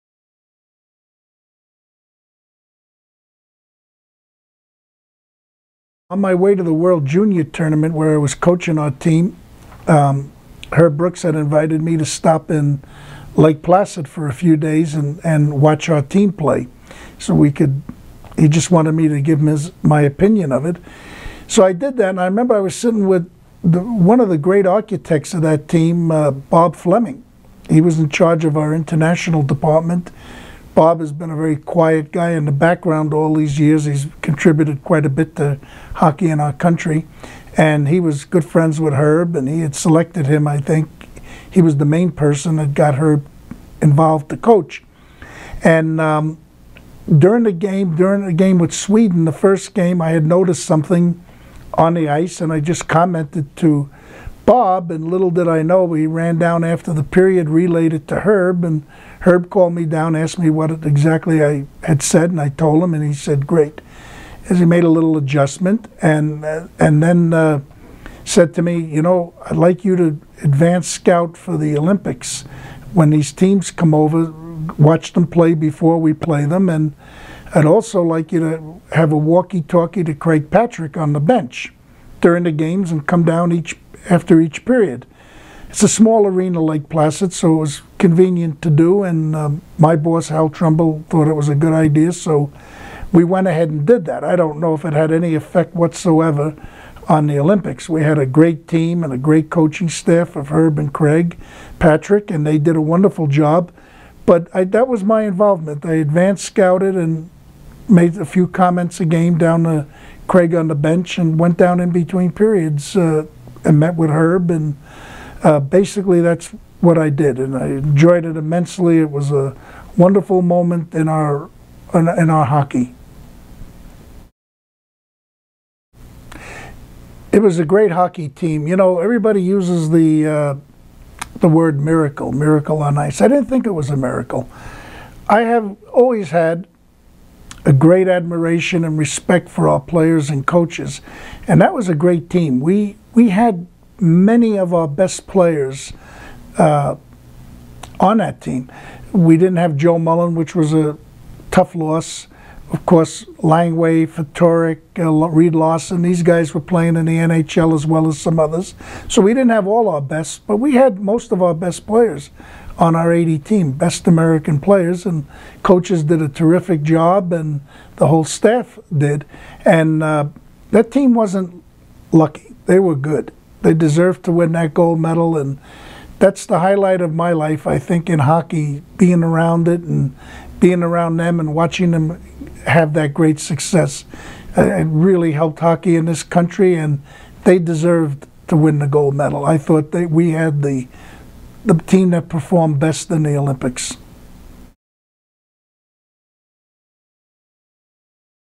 Lunch With Lou - Miracle On Ice
Lunch with Lou: Lou Vairo talks about his involvement with the 1980 Miracle on Ice team. '